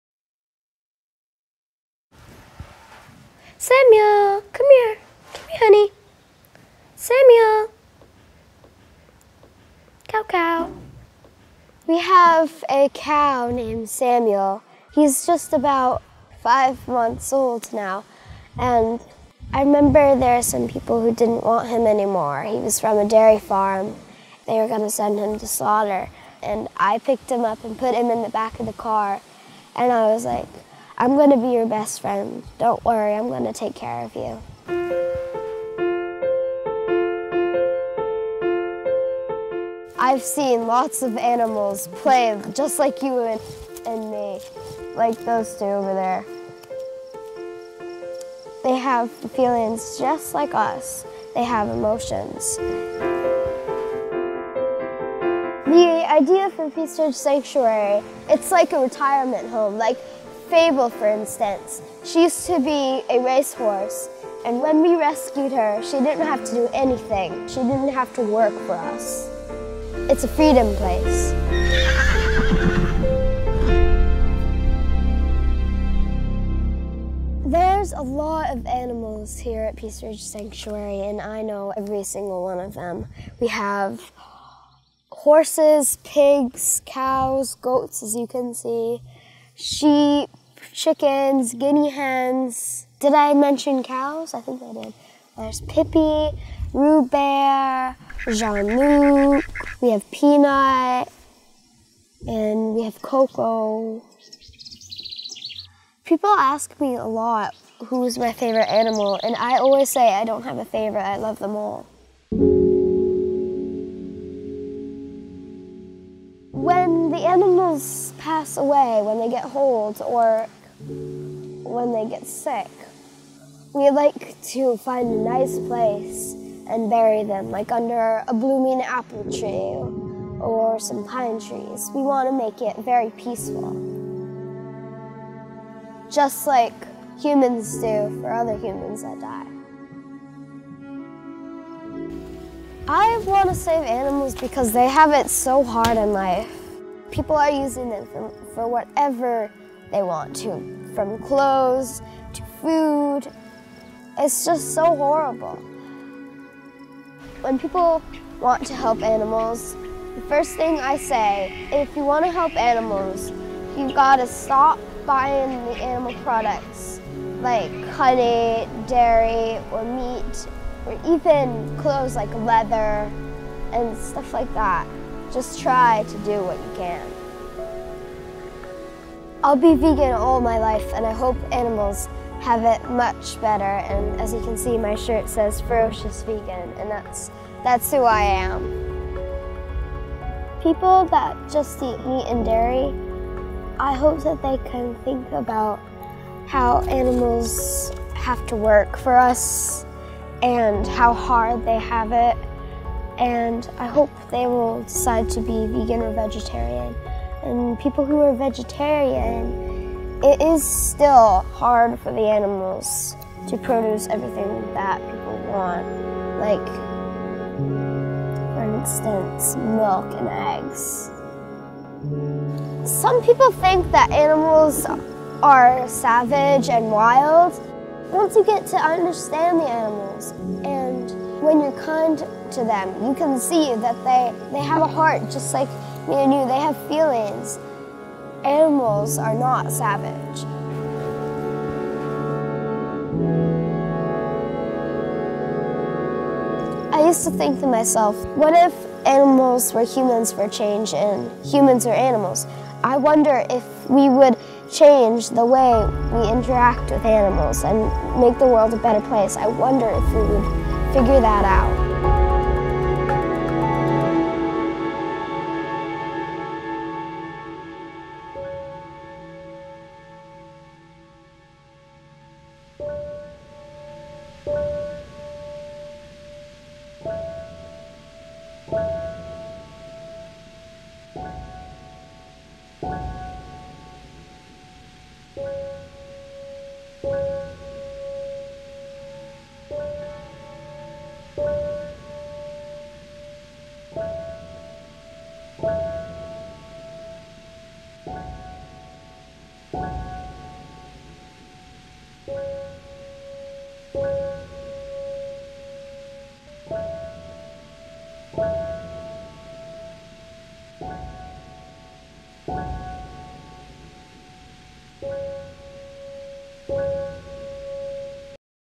aVa short documentary by Luis Carlos Davis
Ava, a 9 year girl saves animals in a animal sanctuary in the state of Maine.